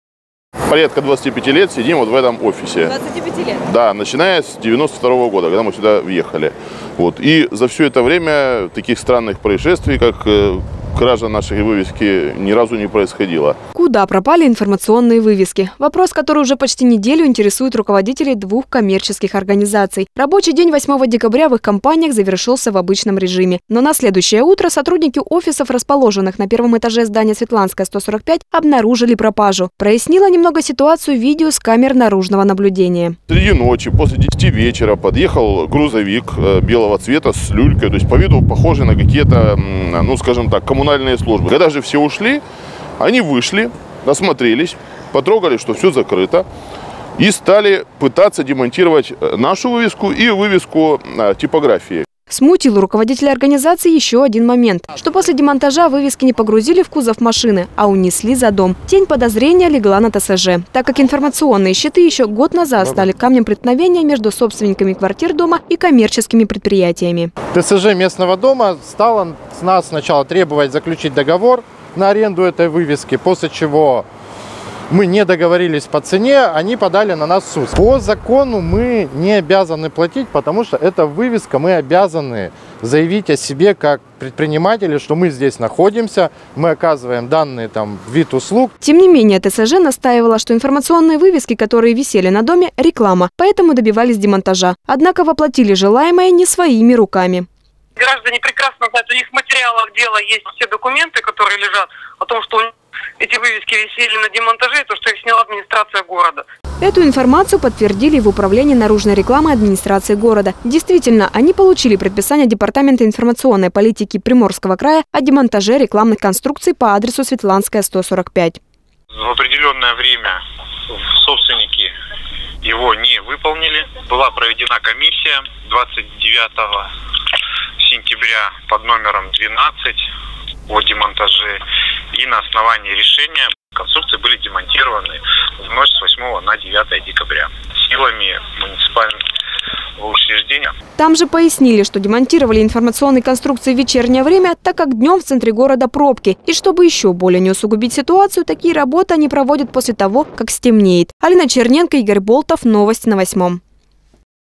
Во Владивостоке стали пропадать информационные вывески
Во Владивостоке под покровом ночи пропадают информационные вывески. Это стало поводом для беспокойства руководителей коммерческих организаций города.